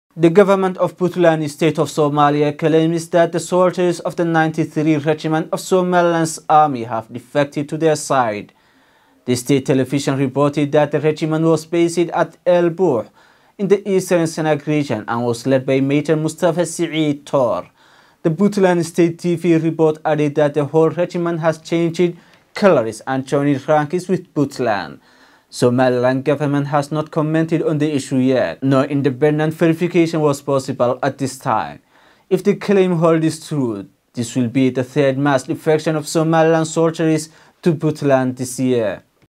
SOMALILAND Soldiers have defected to Puntland CBAtv English
CBAtv English PUNTLAND SAYS SOME SOMALILAND SOLDIERS HAVE DEFECTED TO THEIR SIDE